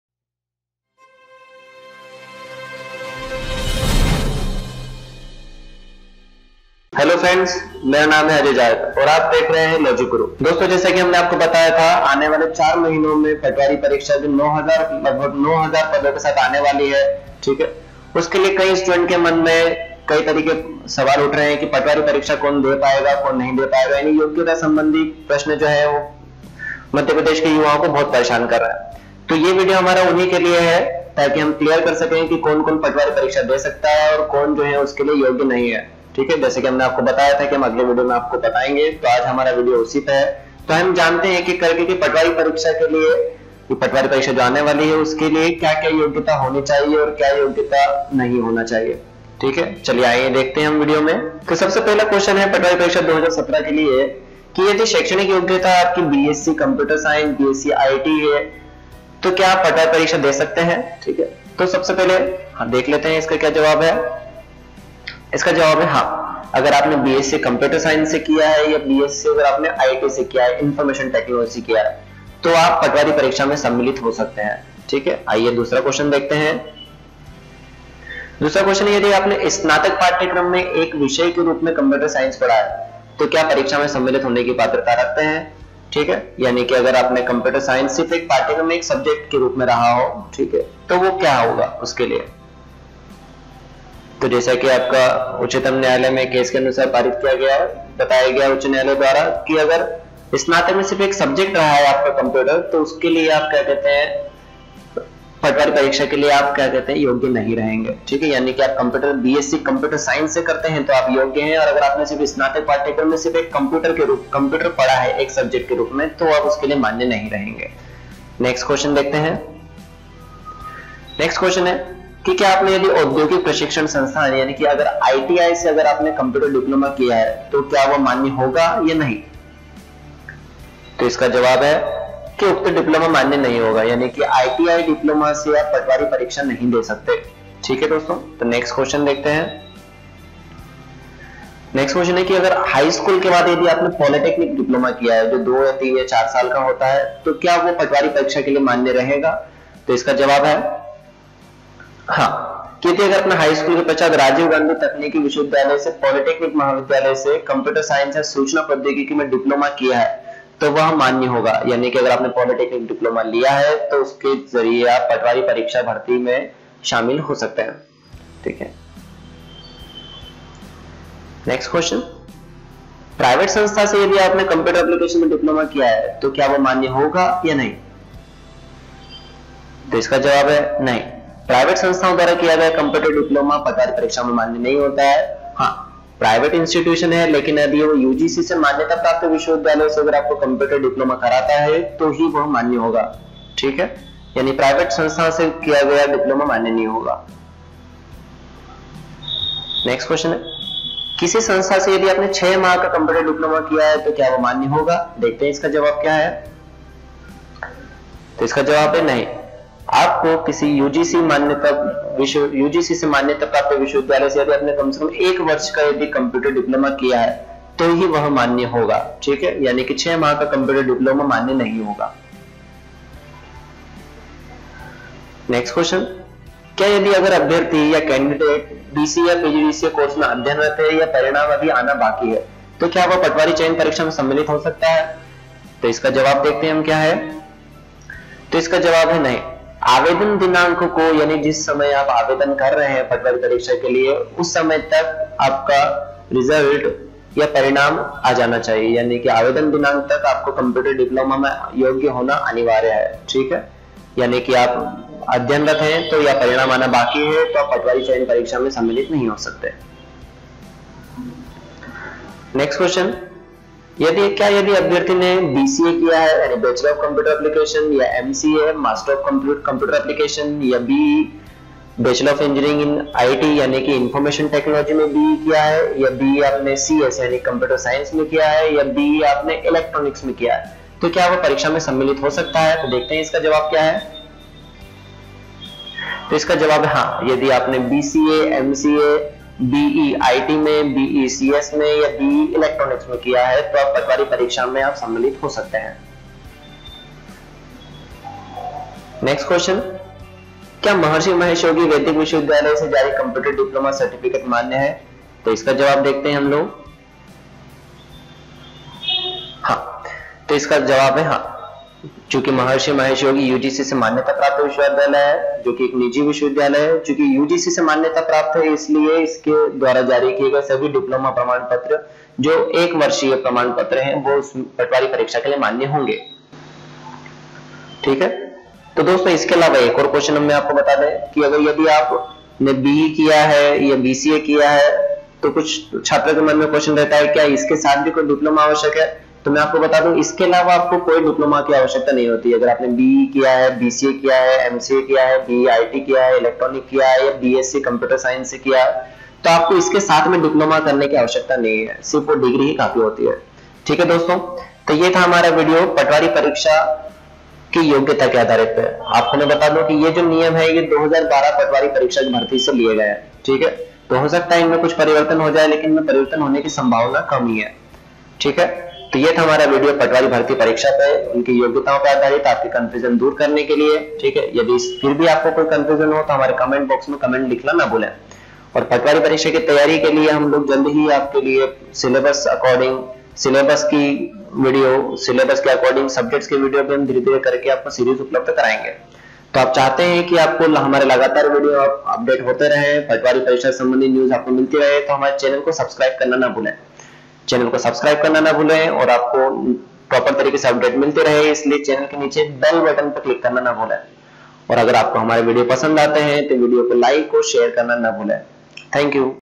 patwari exam 2017- eligibility (मध्यप्रदेश पटवारी परीक्षा 2017- पात्रता)
मध्यप्रदेश पटवारी परीक्षा  के लिये अनिवार्य योग्यता एवं अयोग्यता की जानकारी हेतु इस विडियो को अंत तक अवश्य देखे |